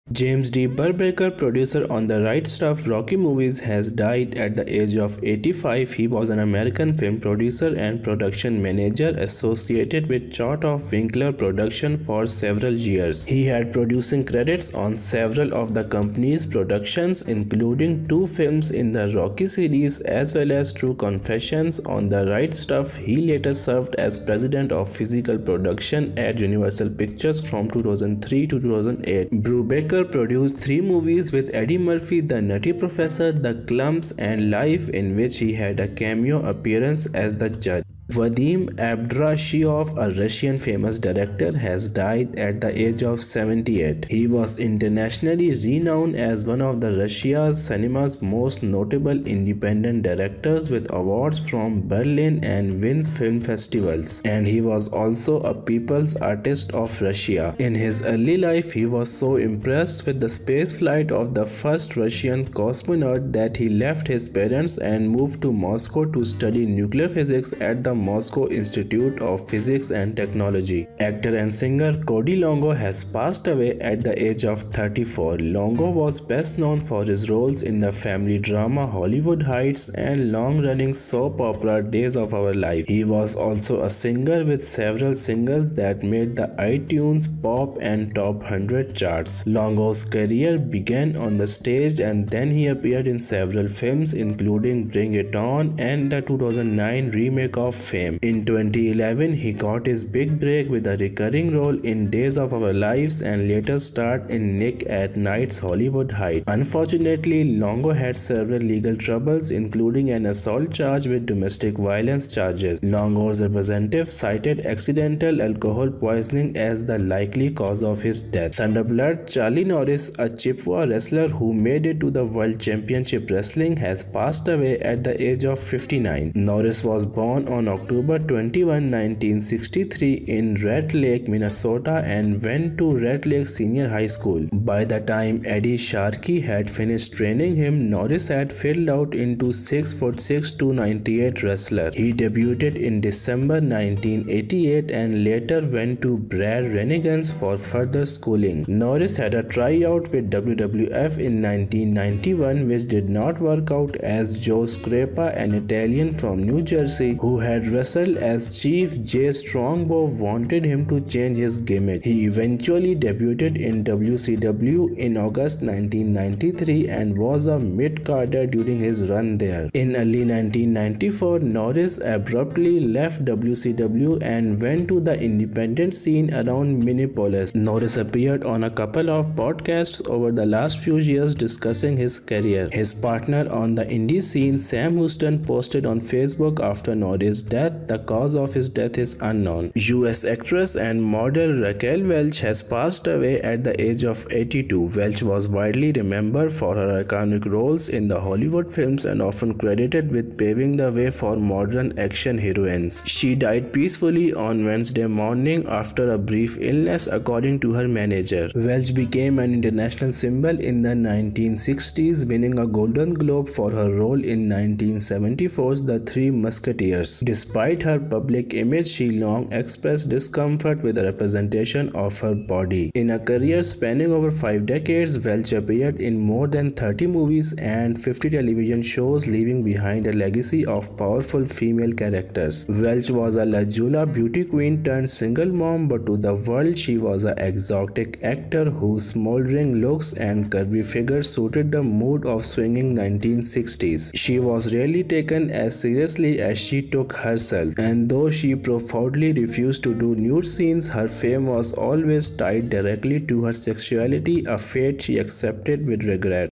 6 Famous Celebrities Who Died Today 26 February 2023
The video is about: 6 Famous Celebrities Who Died Today 19 February 2023

This video explains actors who died today and recent months and year. Actors not alive anymore and Actors who died this February.